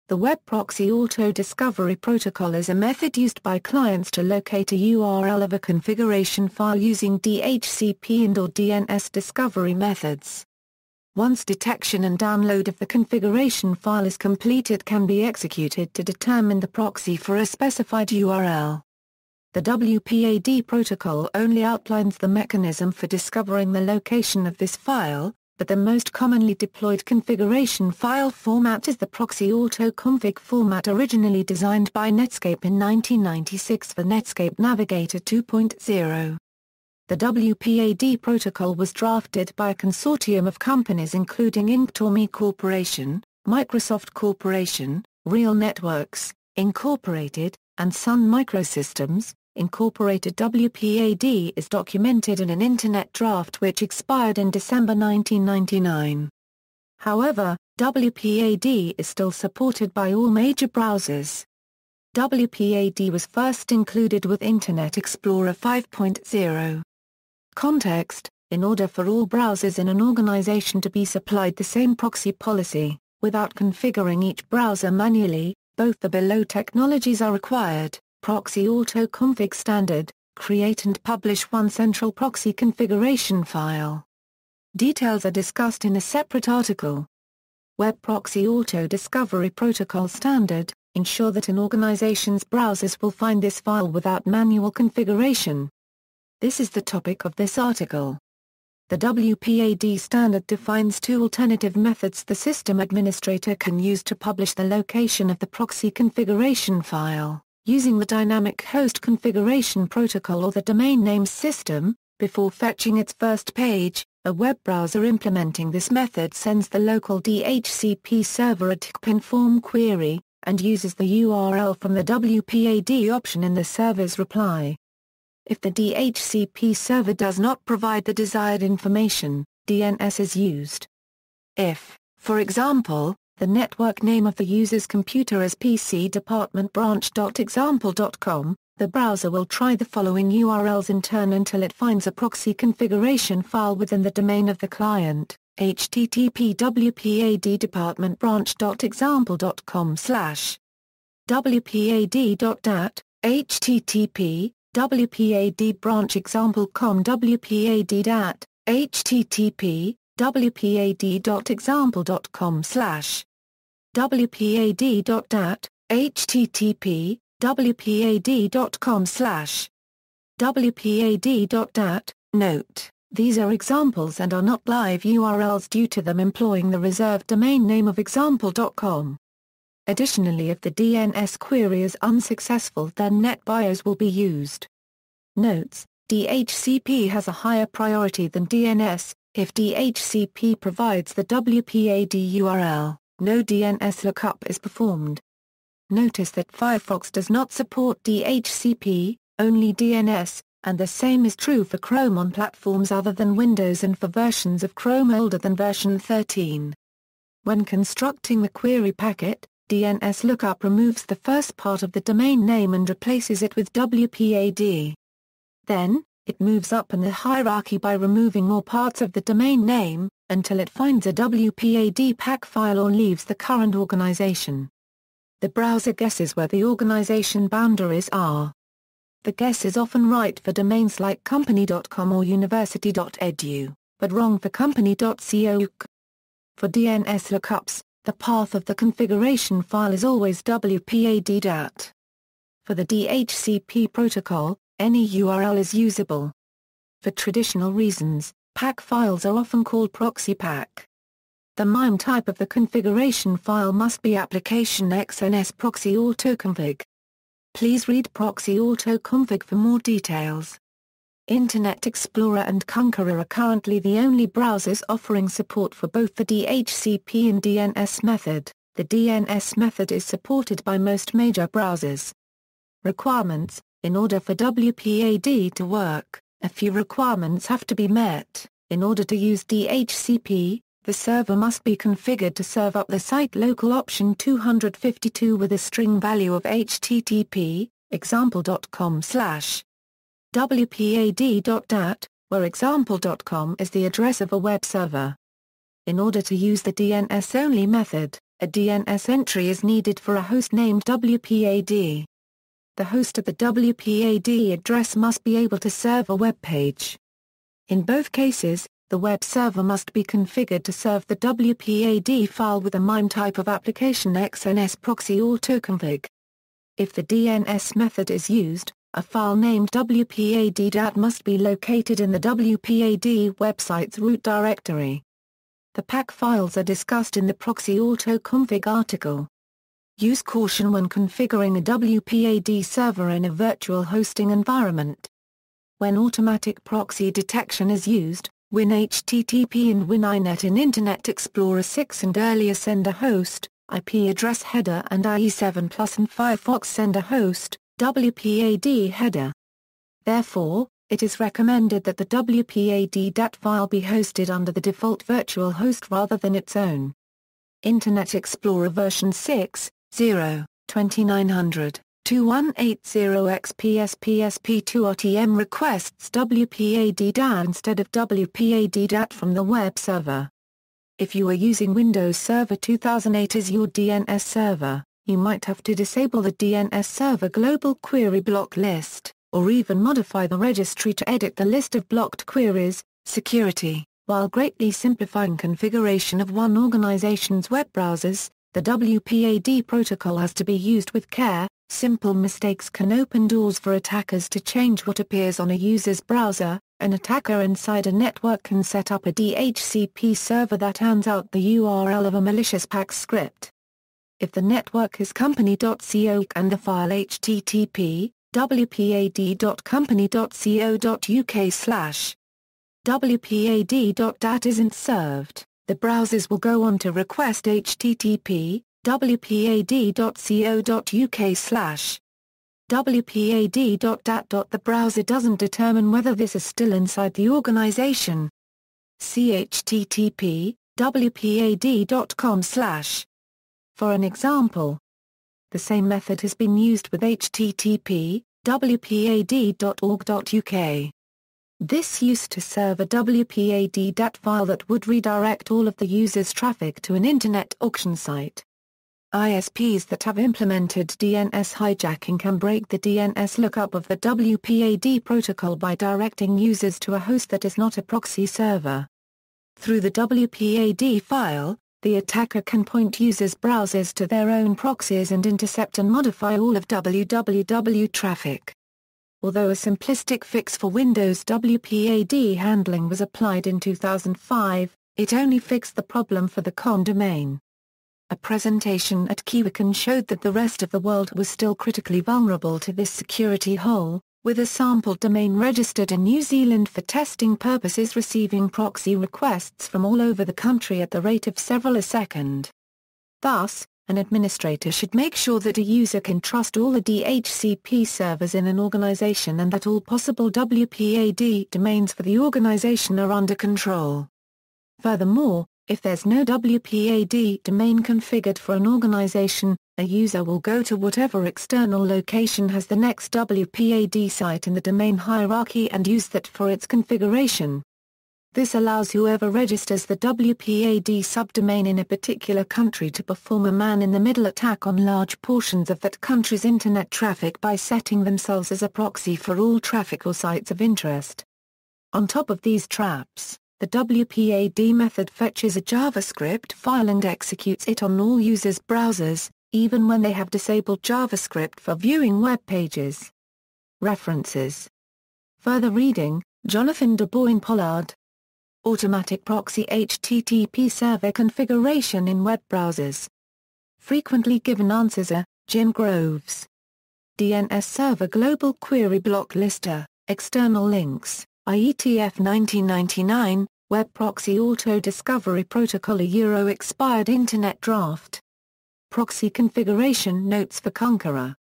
Web Proxy Autodiscovery Protocol
The Web Proxy Auto-Discovery Protocol (WPAD) is a method used by clients to locate a URL of a configuration file using DHCP and/or DNS discovery methods. Once detection and download of the configuration file is complete it can be executed to determine the proxy for a specified URL. The WPAD protocol only outlines the mechanism for discovering the location of this file, but the most commonly deployed configuration file format is the Proxy auto-config format originally designed by Netscape in 1996 for Netscape Navigator 2.0. The WPAD protocol was drafted by a consortium of companies including Inktomi Corporation, Microsoft Corporation, RealNetworks, Inc., and Sun Microsystems, Inc.. WPAD is documented in an INTERNET-DRAFT which expired in December 1999. However, WPAD is still supported by all major browsers. WPAD was first included with Internet Explorer 5.0.